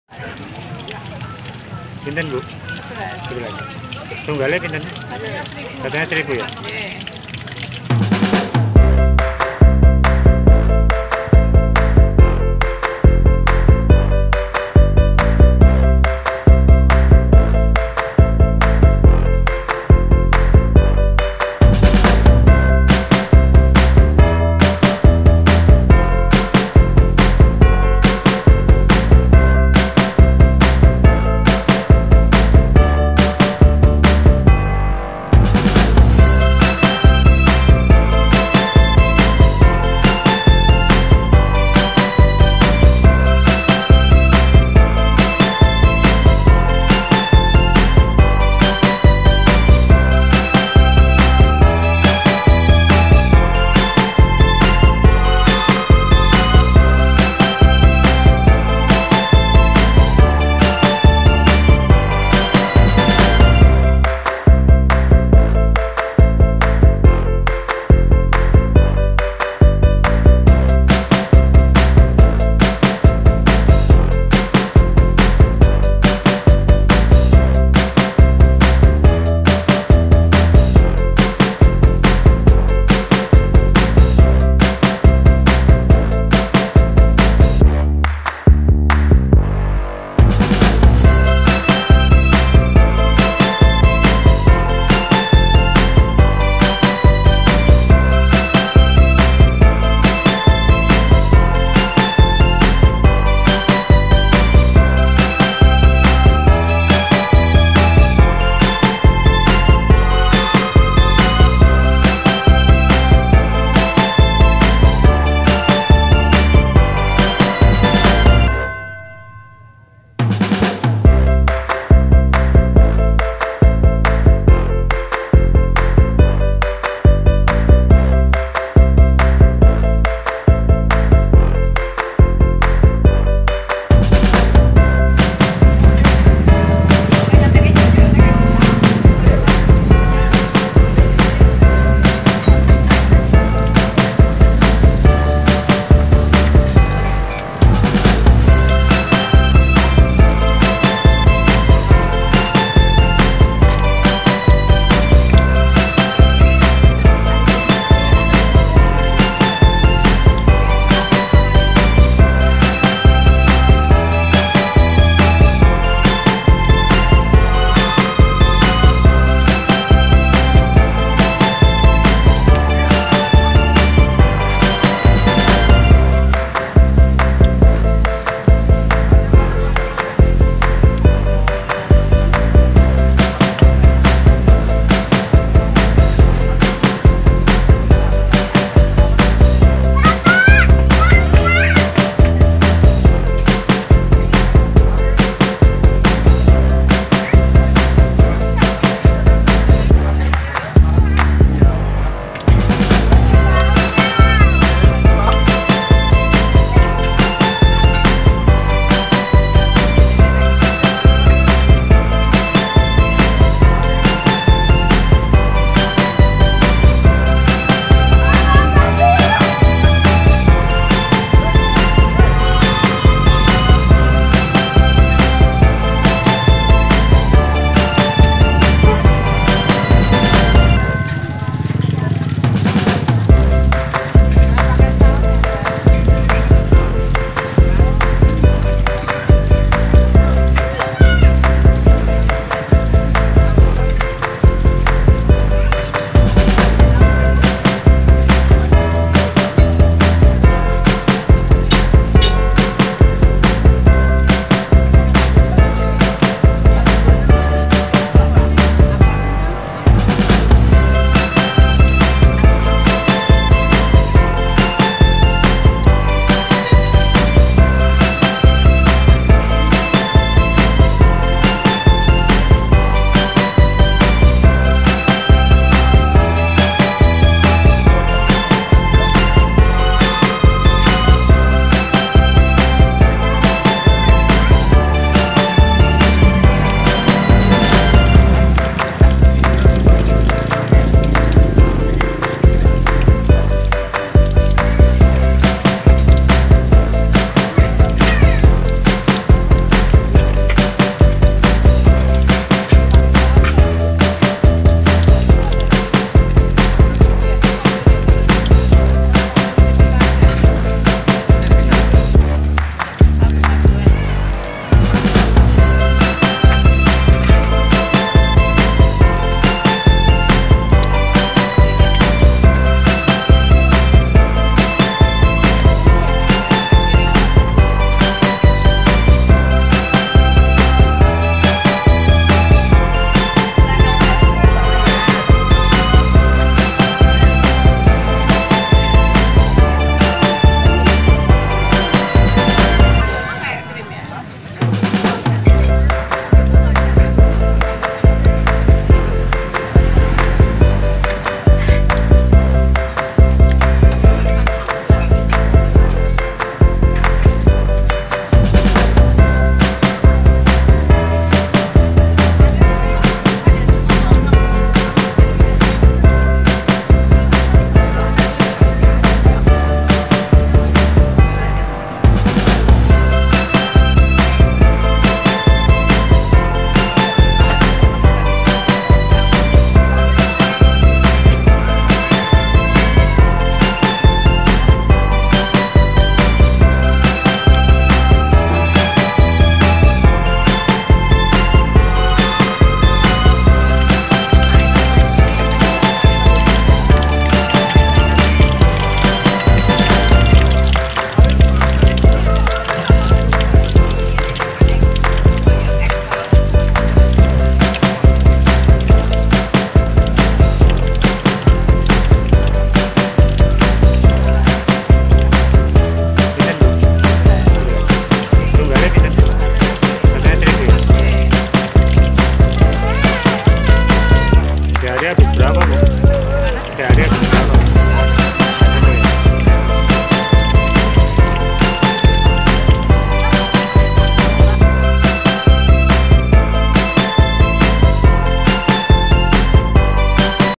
TEMPURANYA ENAK DAN LENGKAP BANGET!!! Tempura Mbak Yayuk Bu Yayuk Alkid Jogja - Kuliner Alkid Jogja
Tempura Mbak Yayuk / Bu Yayuk Alkid merupakan salah satu penjual tempura paling enak dan murah di Jogja. Pilihan tempura di sini bermacam-macam bentuk dan warnanya, dari mulai pipih, lonjong, bulat, bintang, dan lainnya. Setiap pembeli bebas memilih tempuranya lalu akan digoreng. Sebagai pelengkap, tersedia saus untuk dicocol dengan tempura gorengnya.

Lokasi / Alamat : Jl. Alun Alun Kidul, Patehan, Kecamatan Kraton, Kota Yogyakarta, Daerah Istimewa Yogyakarta
Jam Buka : Pukul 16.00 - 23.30 WIB
Harga : Rp.1.000,- per buah